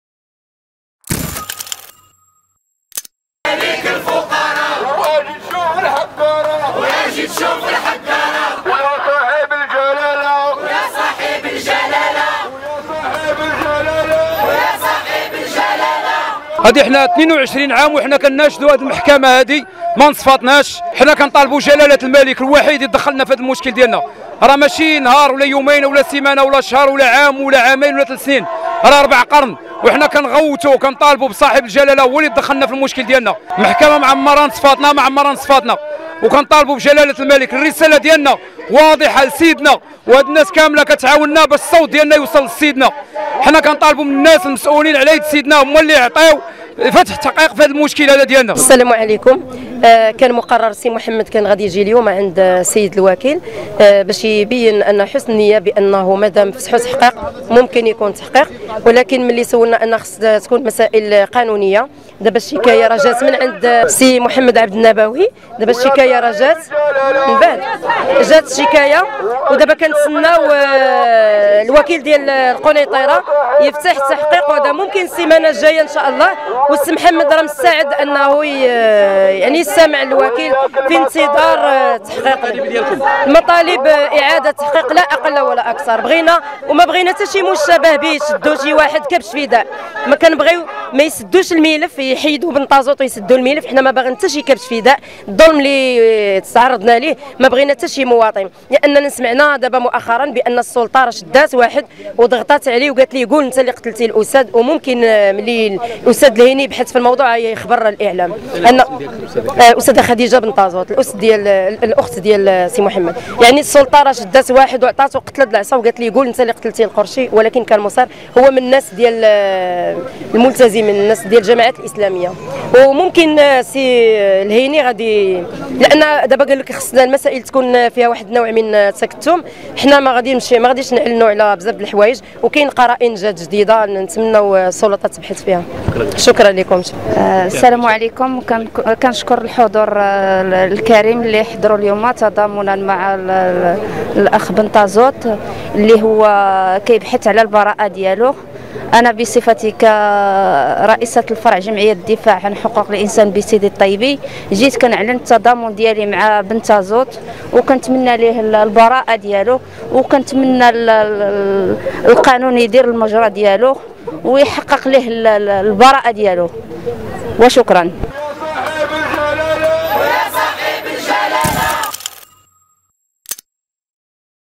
تصريح صــادم للأستاذة أخت بنطازوط من أمام محــكمة الإســتئناف بالقنيطرة
Alwankoum
فيديوهات مغربية
صوت وصورة